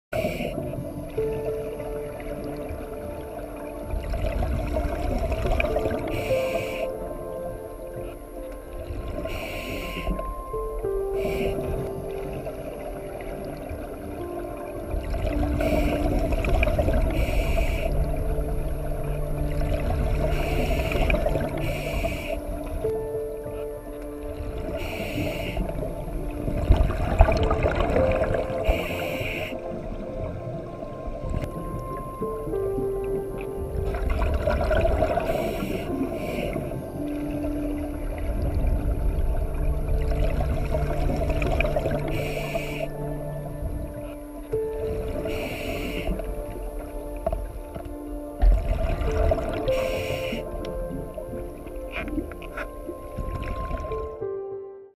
Planktonic Bait Ball At Ogden Point
A Planktonic Bait Ball suns itself in the shallows off Ogden Point